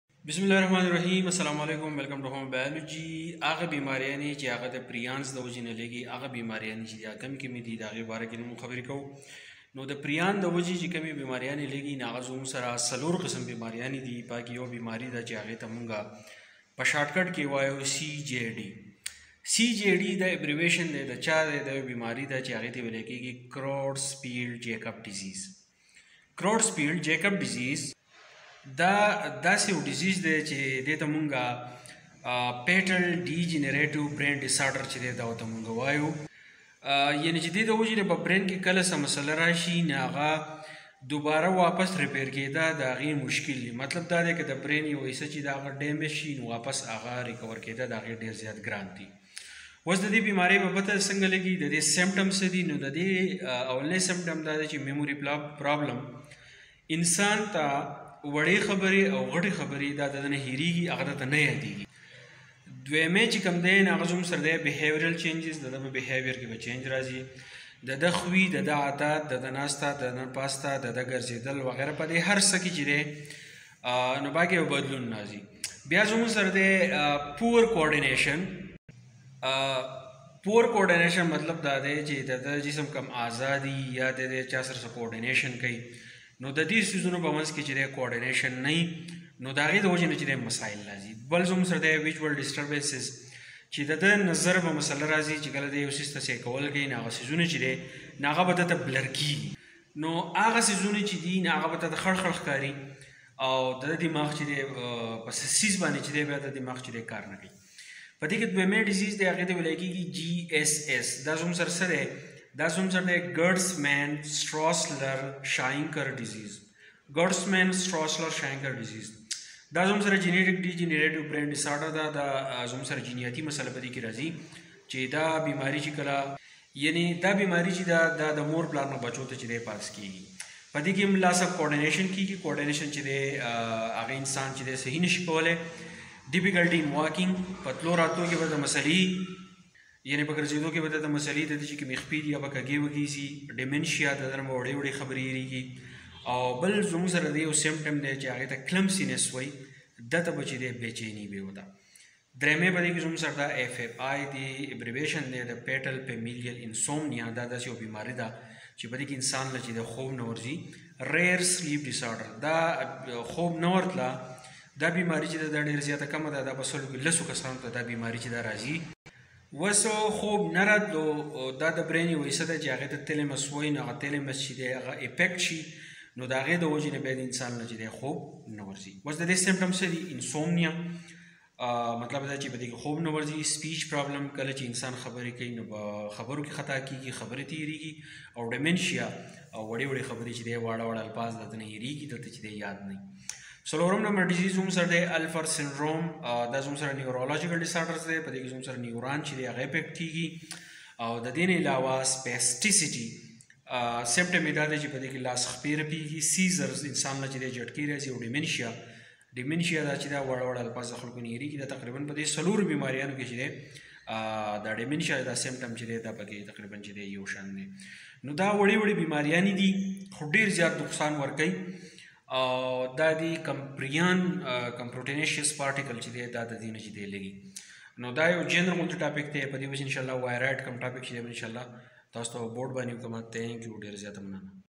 Diseases caused by prions | CJD, GSS, FFI, Alpers syndrome
In this lecture you will know about the diseases caused by prions in pashto. These diseases are CJD ( Creutzfeld jacob disease), GSS (Gertsman straussler schienker) , FFI (Fatal familial insomnia), Alpers syndrome. This topic has been taken from class 11 biology